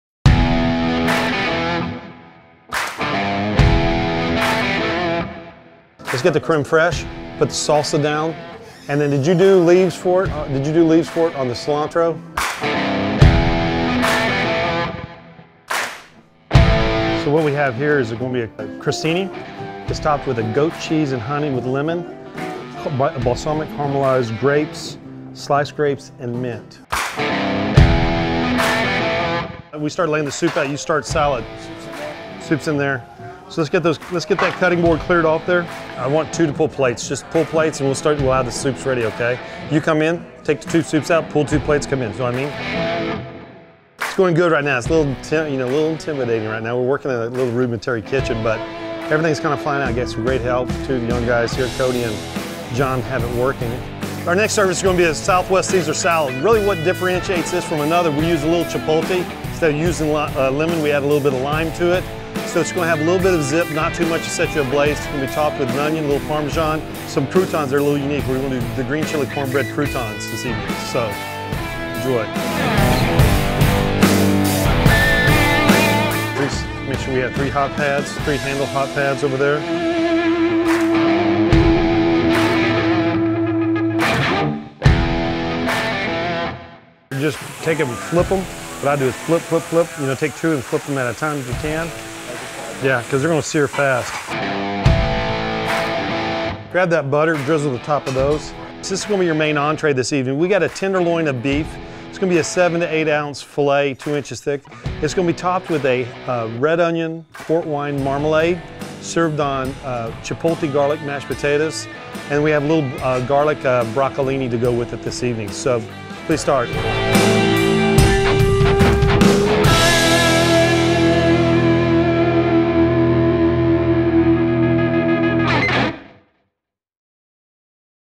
Cowboy Chef Newton Event Opportunities
From dinner parties to weddings and corporate events, how can Chef Newton take your next event from ordinary to extraordinary?